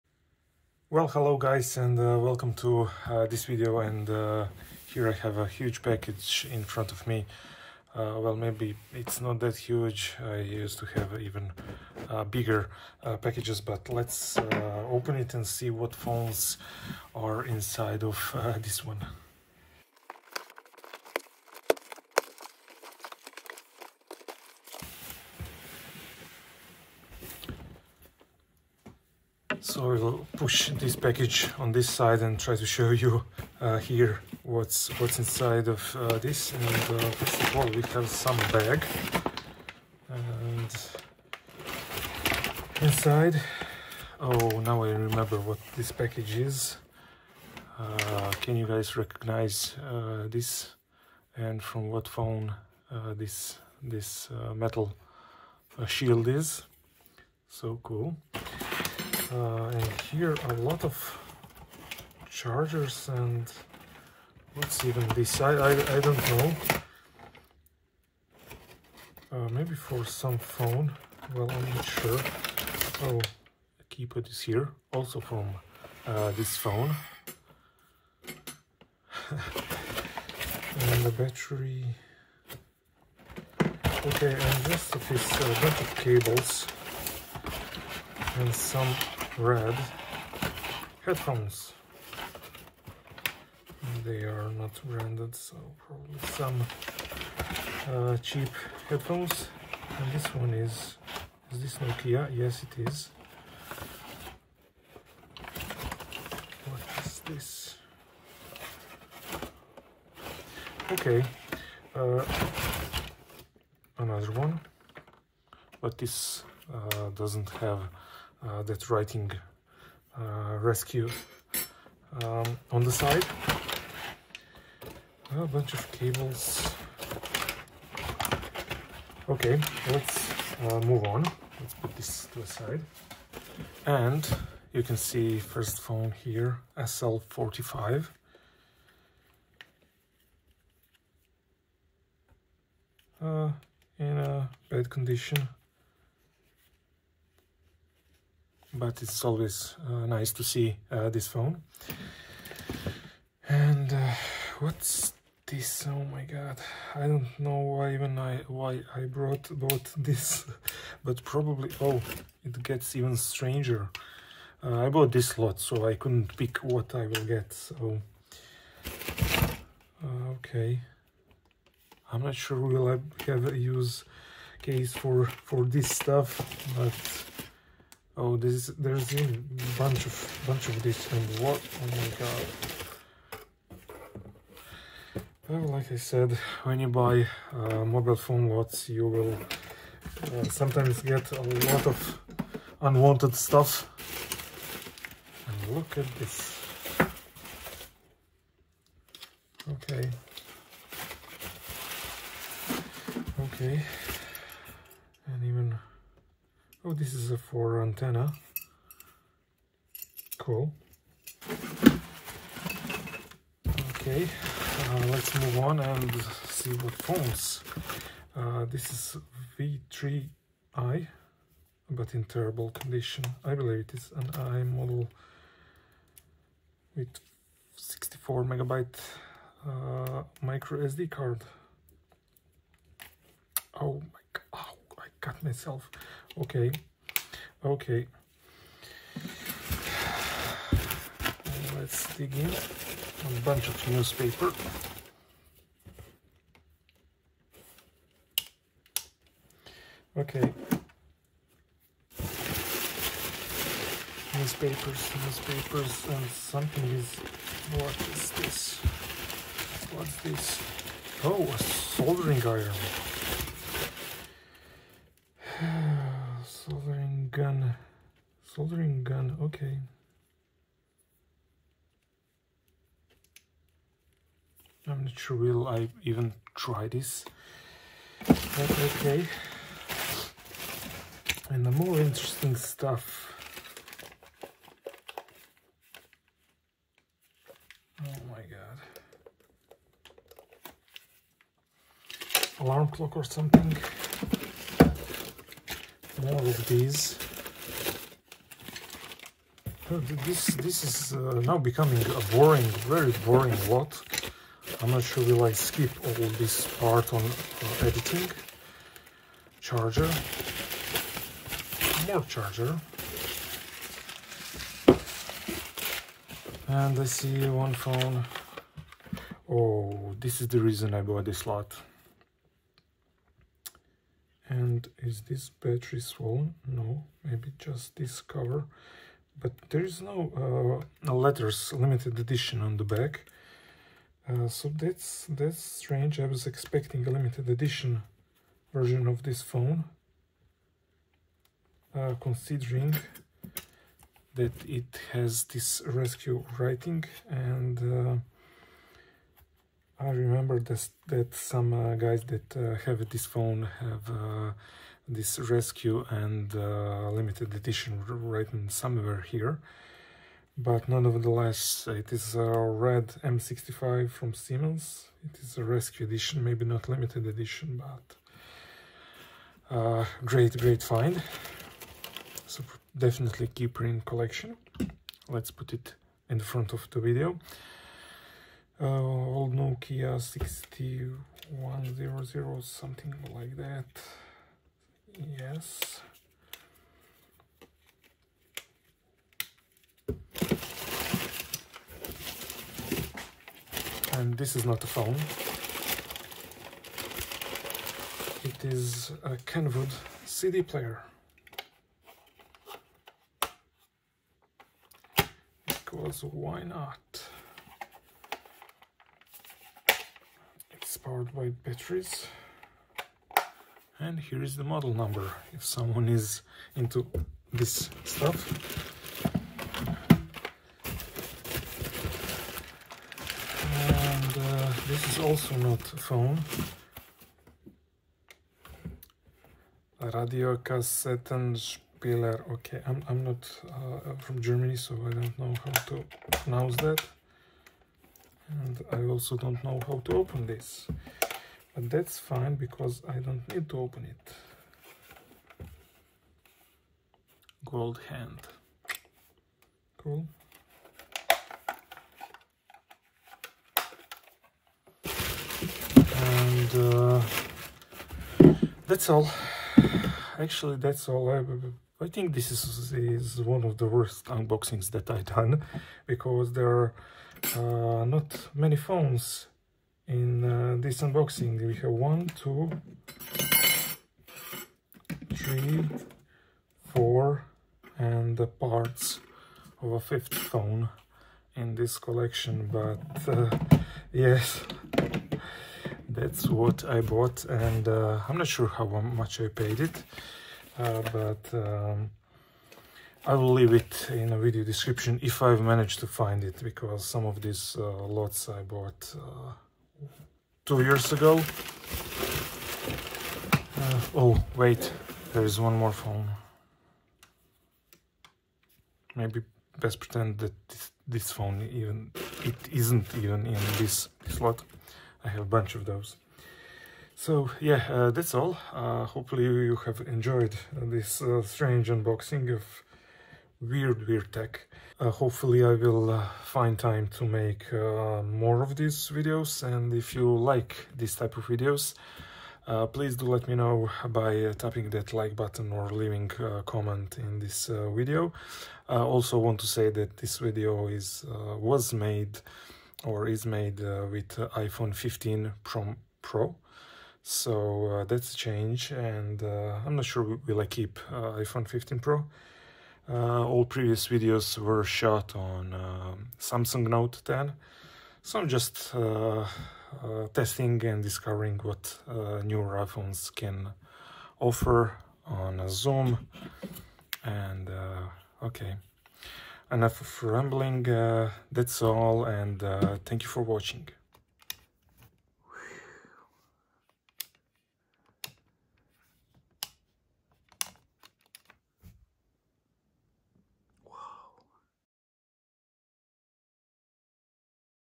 Old Tech and Other eBay Junk
Another eBay junk lot unboxing with one interesting phone inside, an Siemens M65 in red color "RESCUE" Edition.